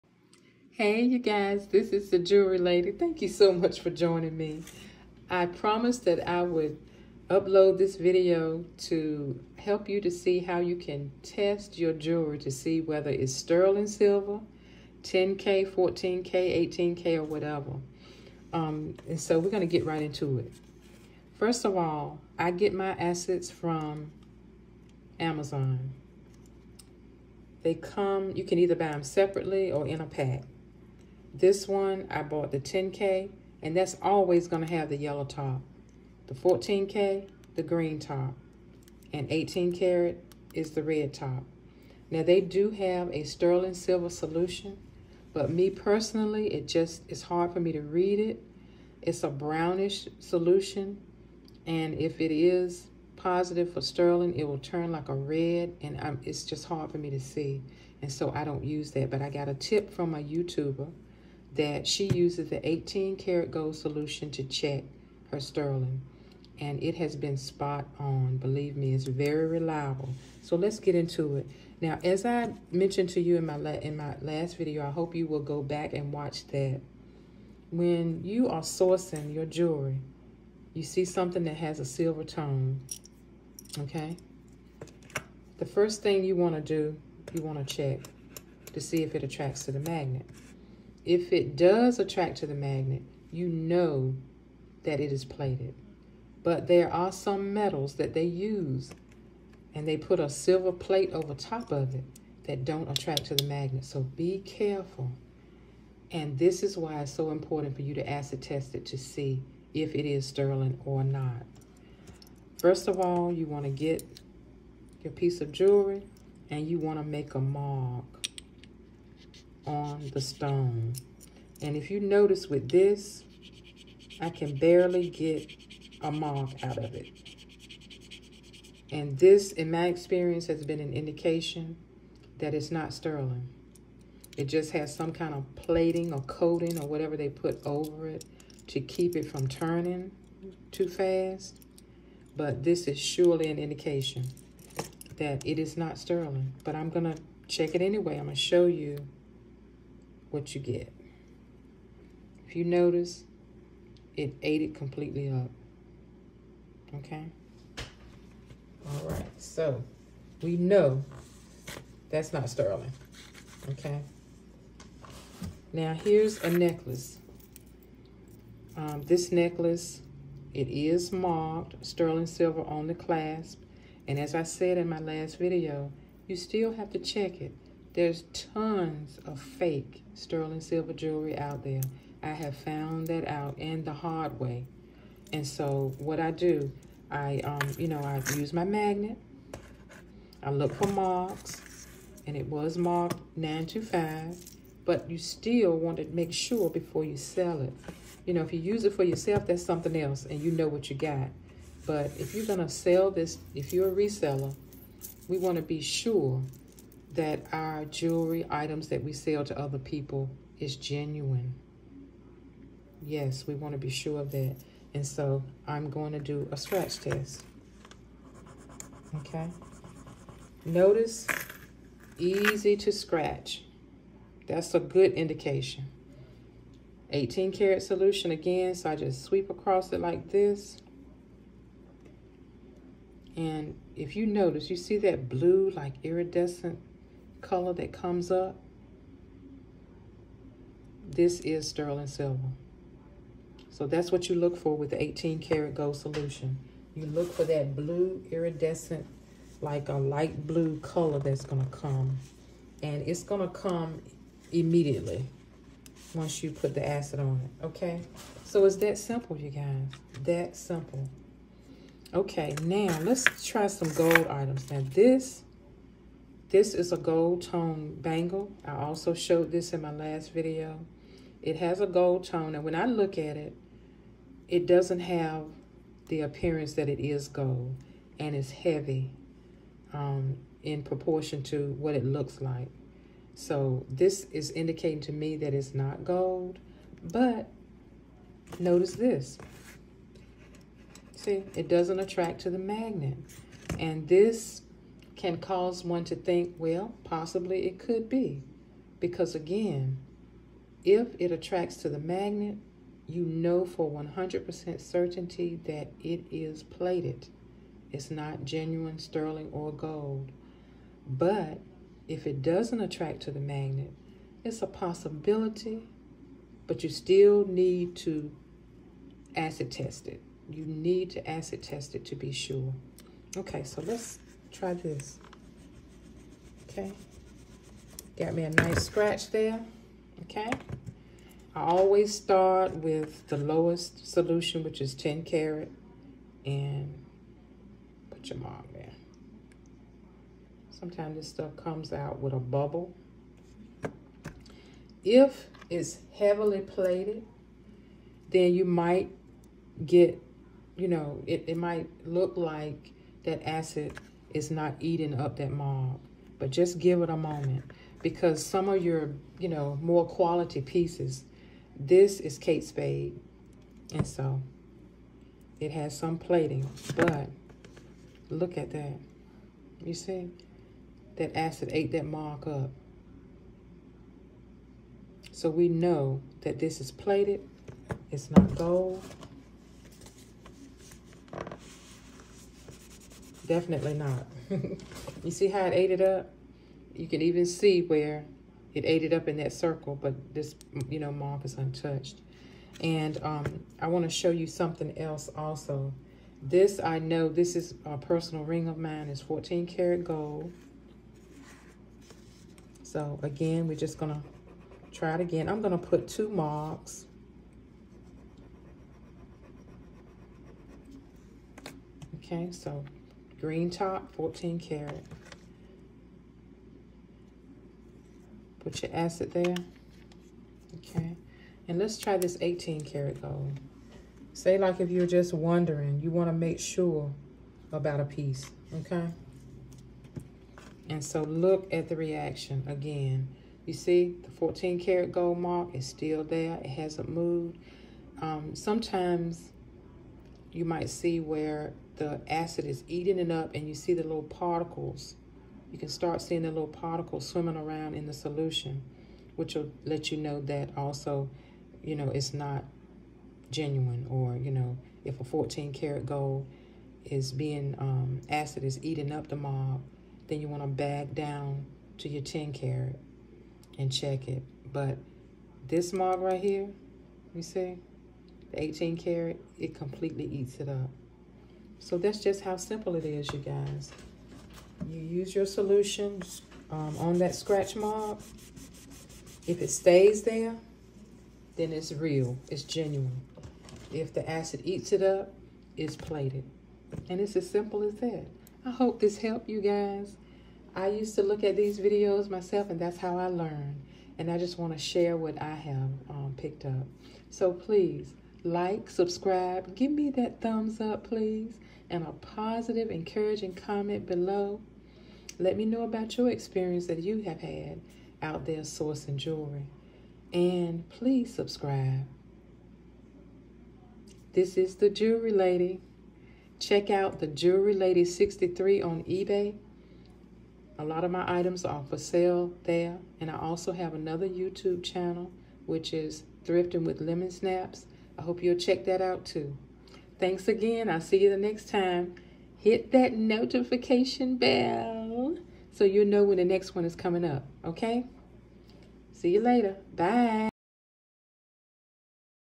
HOW TO ACID TEST YOUR JEWELRY | IS IT 10K? 14K? STERLING? | thejewelrylady63 EBay
Learn how easy it is to acid test your jewelry at home.
You can find out whether it’s 10k 14K 18K or sterling silver on your own.